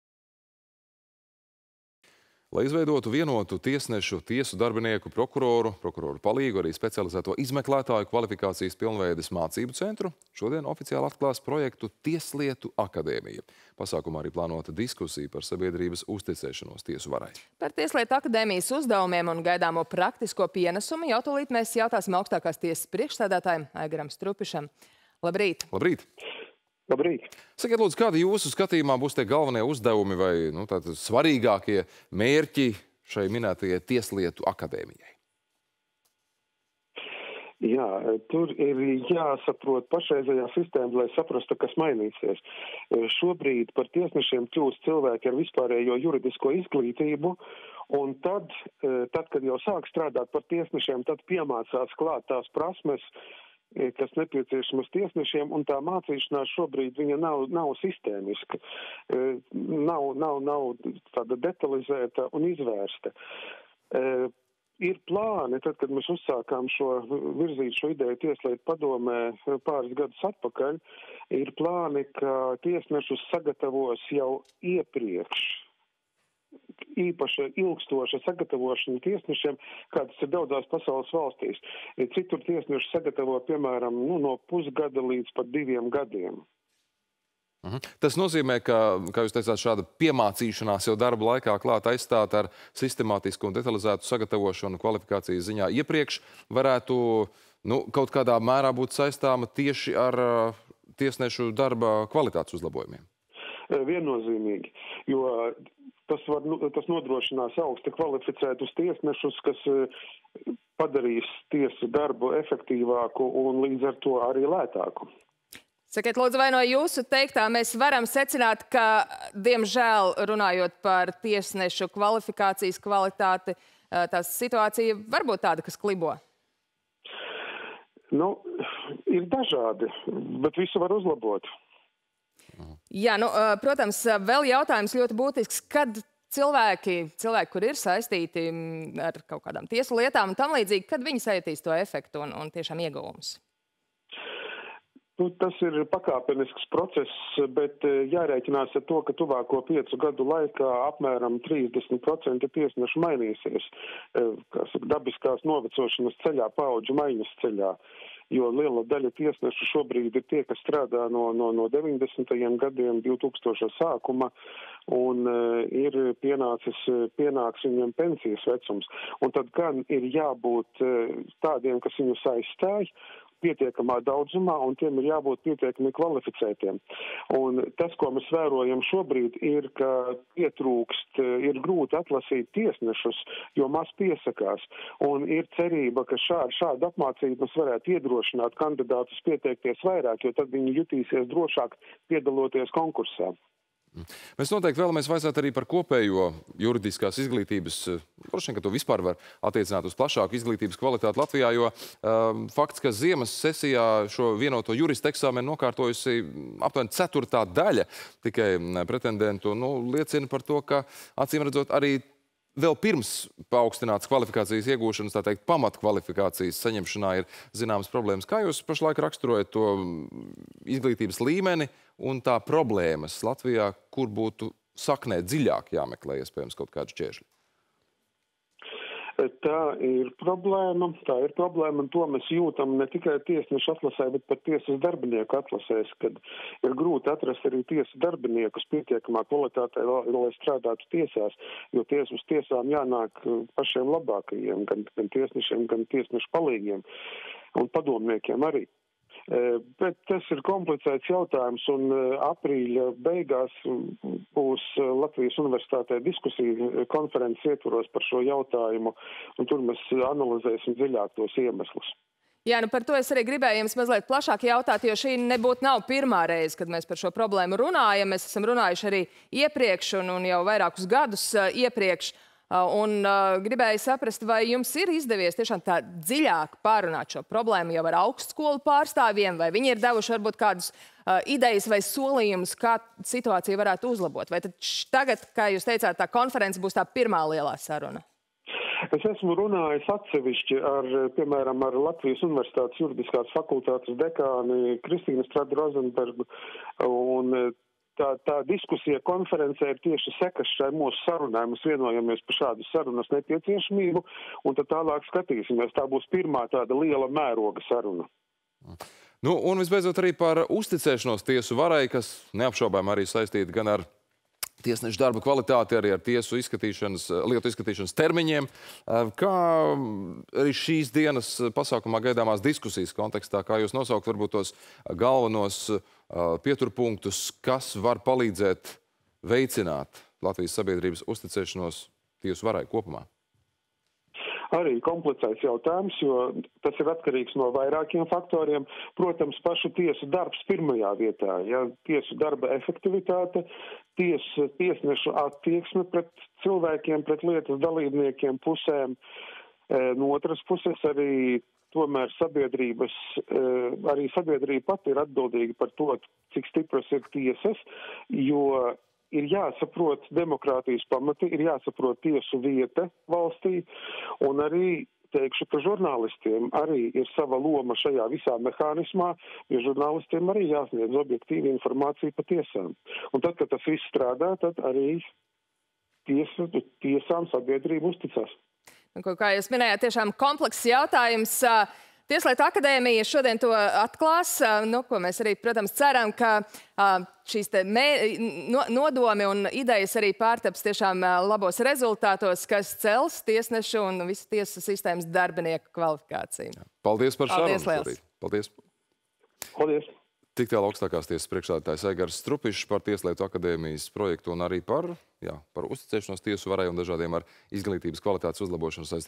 Telefonintervija ar Augstākās tiesas priekšsēdētāju Aigaru Strupišu
Lai izveidotu vienotu tiesnešu, tiesu darbinieku, prokuroru, prokuroru palīgu un specializēto izmeklētāju kvalifikācijas pilnveides mācību centru, šodien oficiāli atklās projektu “Tieslietu akadēmija”. Pasākumā arī plānota diskusija par sabiedrības uzticēšanos tiesu varai.
      Par Tieslietu akadēmijas uzdevumiem un gaidāmo praktisko pienesumu jau tūlīt jautāsim Augstākās tiesas priekšsēdētājam Aigaram Strupišam.